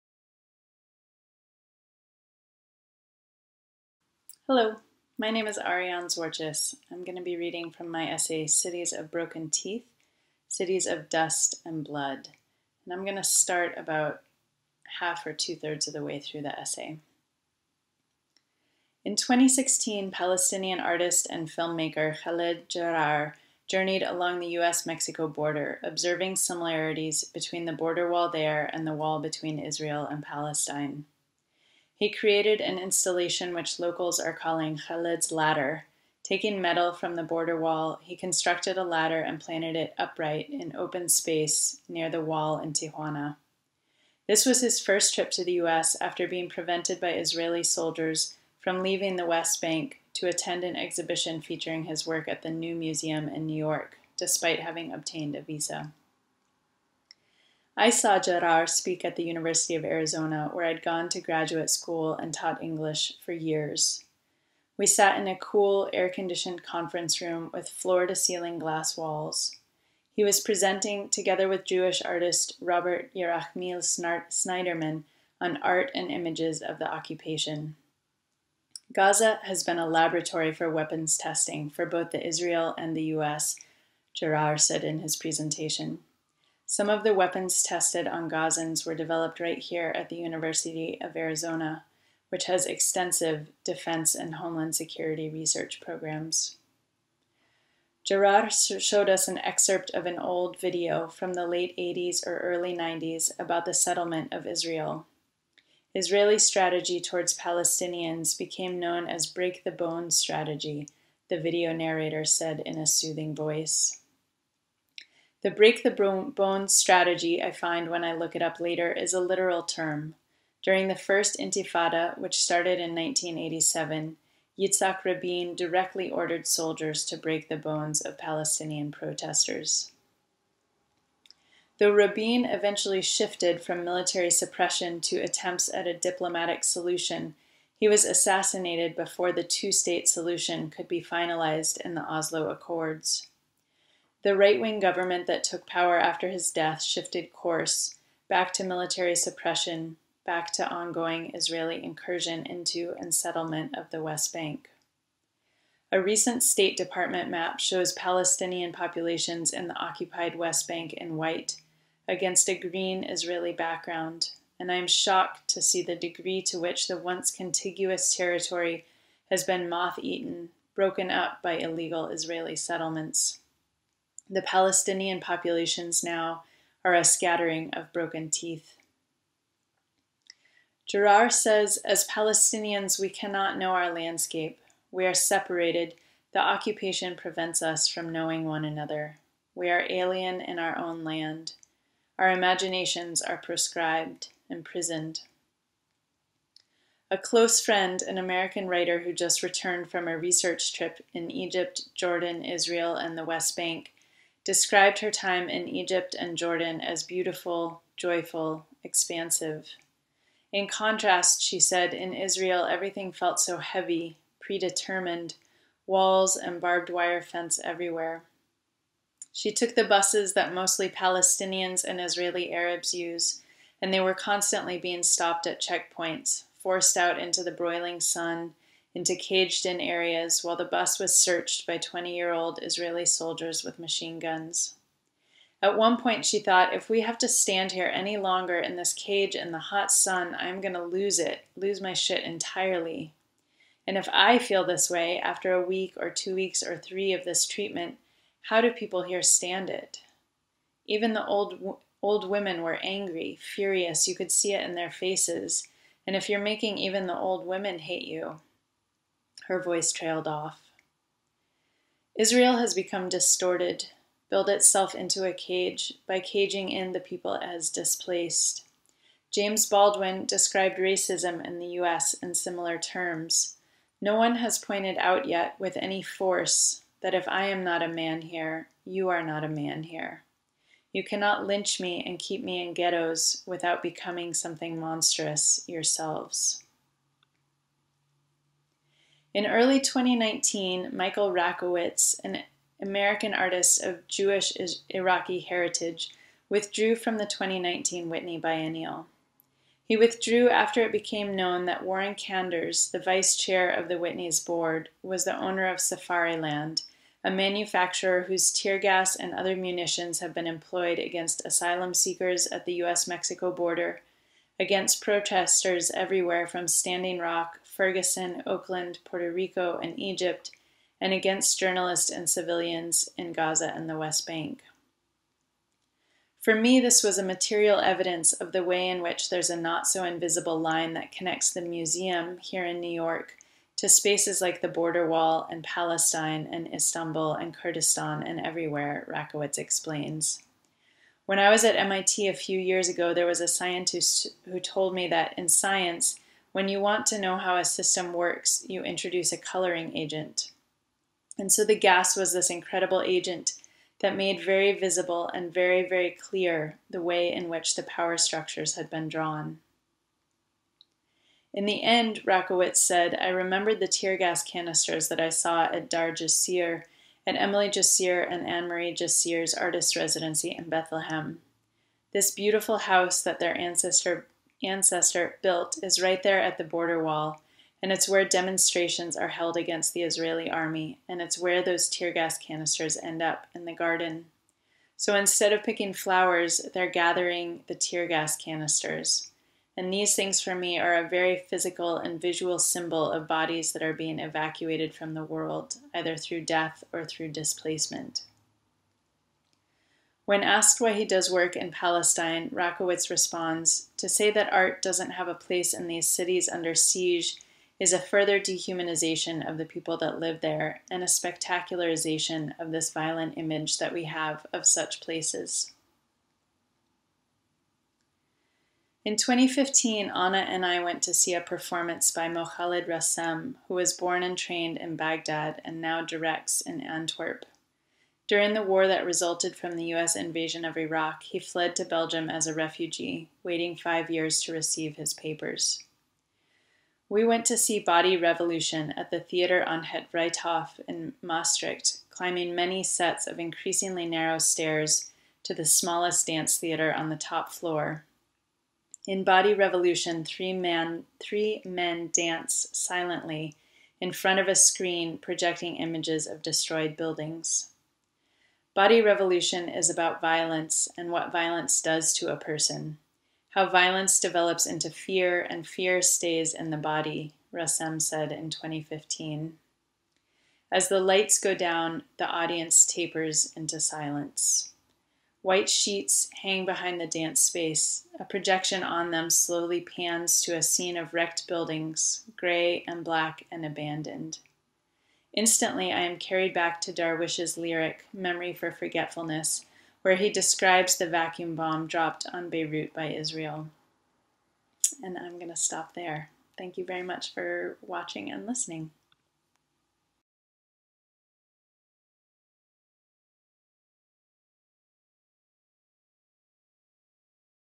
Witness Reading by Arianne Zwartjes
Arianne Zwartjes reads from her essay, "Cities of Broken Teeth, Cities of Dust and Blood" from Witness Magazine's Fall/Winter 2020 Issue.

Our readers were drawn into this thought-provoking and lyric work in which Zwartjes explores devastation in the wake of human cruelty and violence in times of war. It is an artful composition at a heartbreaking timbre; we hope you settle in for this compelling look at what remains.

To read this story in its entirety, or to find more work from the issue, visit us

Author Bio: Arianne Zwartjes teaches for Sierra Nevada University’s low-residency MFA program. In her other life she has worked as a wilderness-medicine instructor, an EMT, and a carpenter. She is the author of the lyric nonfiction, medical-humanities book *Detailing Trauma: A Poetic Anatomy*. Her writing won the 2011 *Gulf Coast* Nonfiction Prize, was a *Best American Essays* Notable Essay, and has appeared in *Tarpaulin Sky*, *Kenyon Review*, *The Southern Review*, and other publications. Find her at ariannezwartjes.com.